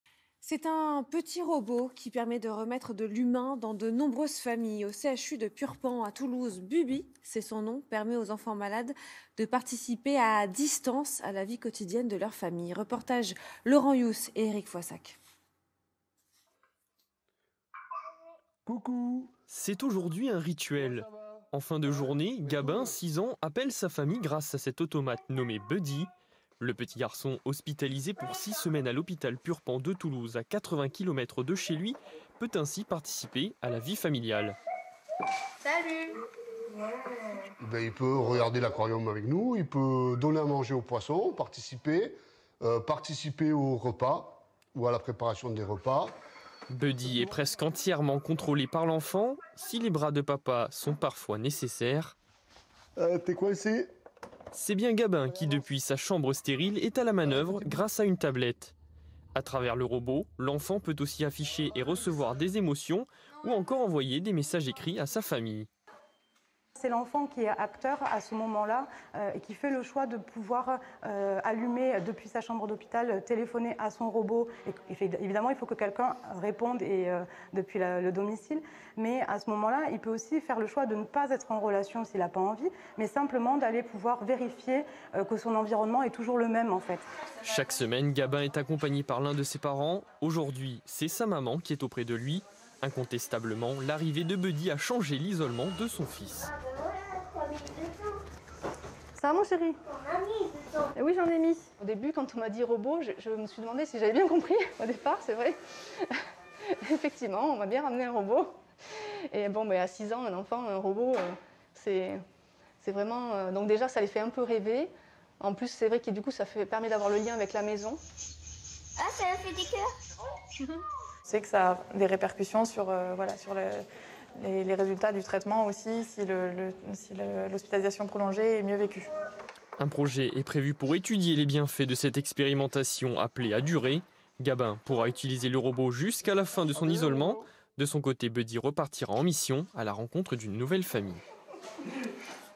Buddy, the Emotional Robot, Helps Sick Kids Cope with Isolation ! 👩‍👩‍👧‍👦❤️
No more feeling of isolation with this companion!
Buddy allows hospitalized children to participate remotely in the daily life of their families. This initiative has beneficial effects on healing.

Fini le sentiment d'isolement avec ce compagnon !
Buddy permet aux enfants hospitalisés de participer à distance à la vie quotidienne de leur famille. Cette initiative a des effets bénéfiques sur la guérison.